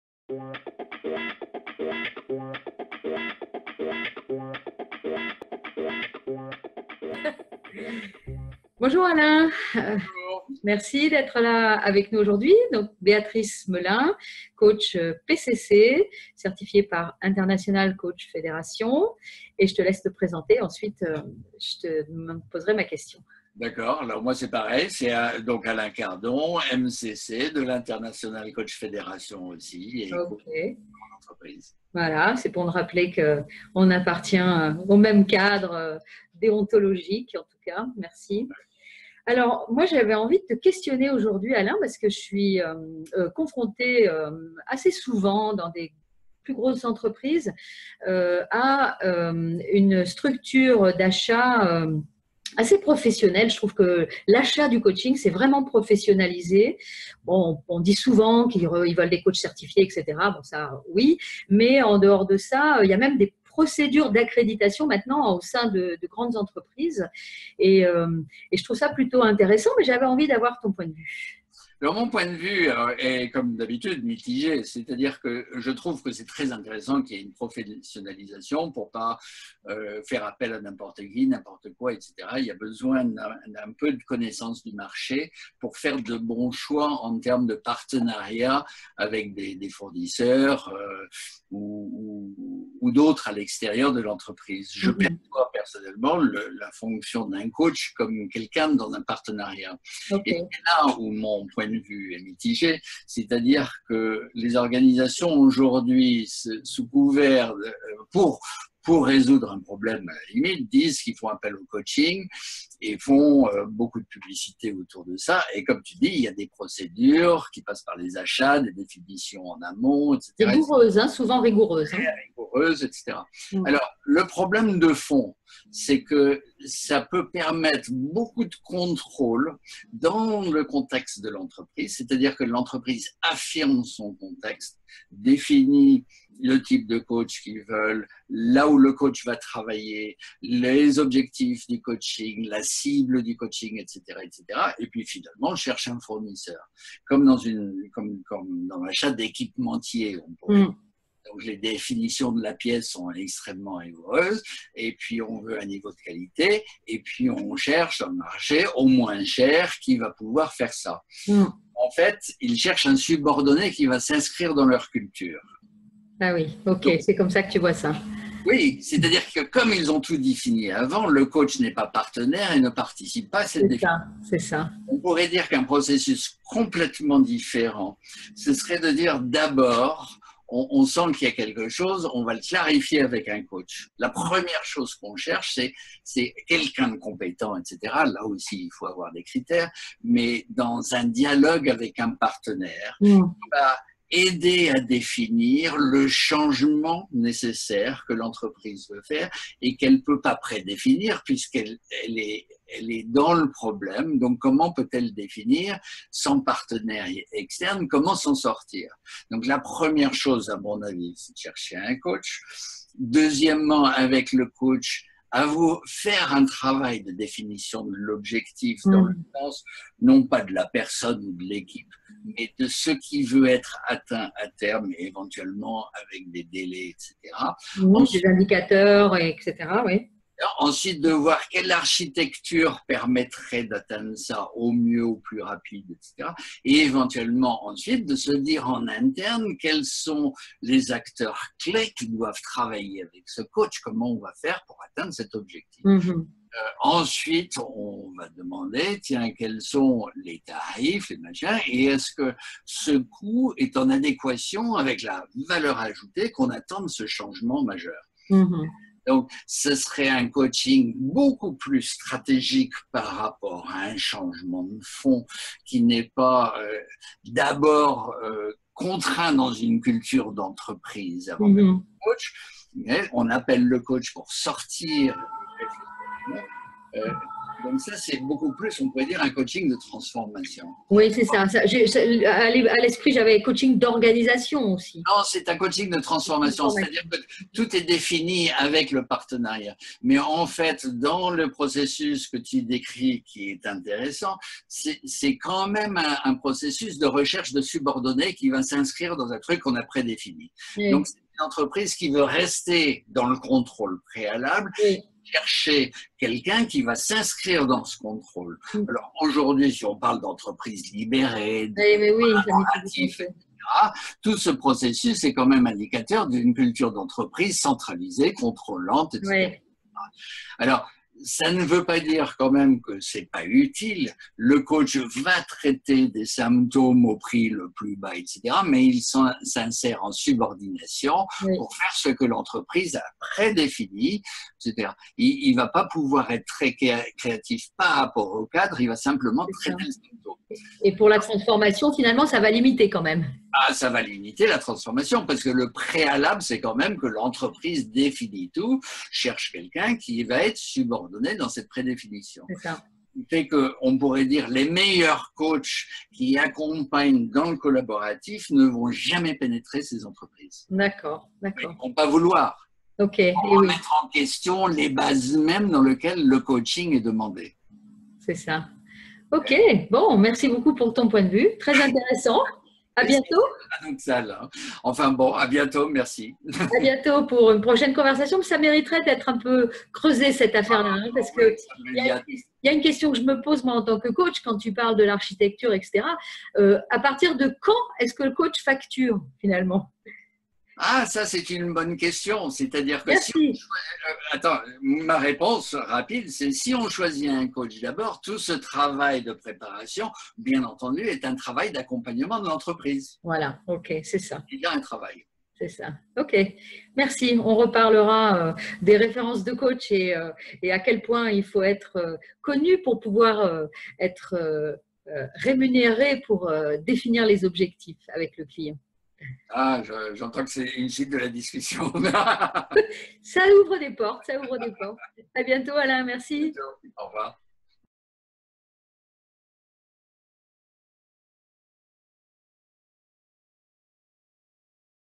Le coach partenaire, et le coach subordonné.
Réflexion sur les conséquences possibles des multiples procédures d'achats voire de validation effectuées par les entreprises clientes de coachs.